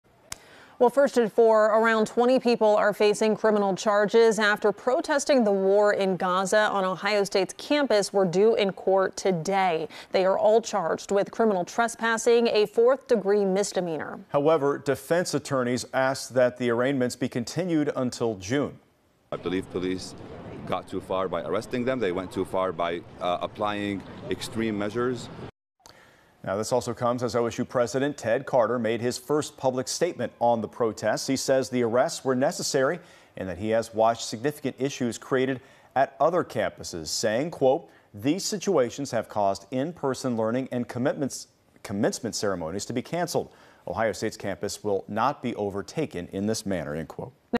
Ohio State president says protest arrests were necessary for safety
As about 20 people facing criminal charges after protesting Israel’s war in Gaza on Ohio State University’s campus were due in court Monday morning, Ohio State’s president stood behind the arrests and the city attorney’s office said it was still deciding whether to move forward with prosecutions.